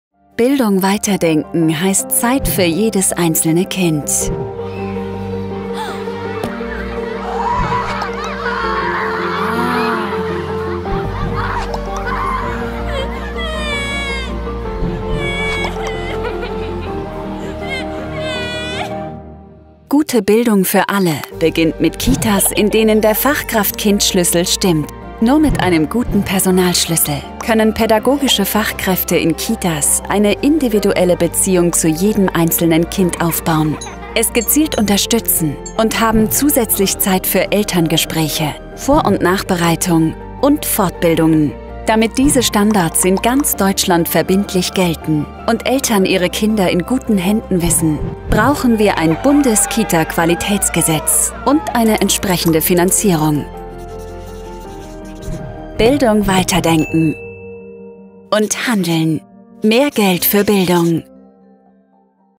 Zeit für jedes einzelne Kind - Bildung. Weiter denken!
Zeit für jedes einzelne Kind, darauf kommt es in den Kitas an. Das geht aber nur, wenn der Fachkraft-Kind-Schlüssel stimmt. Eine gute frühkindliche Bildung gibt es nicht umsonst. Mehr Geld für Bildung!